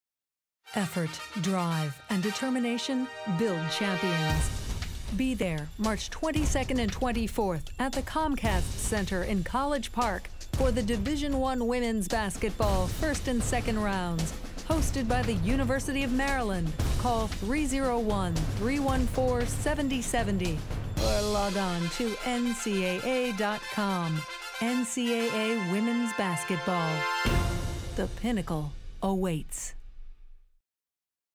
NCAA Division I Women's Basketball 1st/2nd Rounds
2009 NCAA Division I Women's Basketball 1st/2nd Rounds in College Park, MD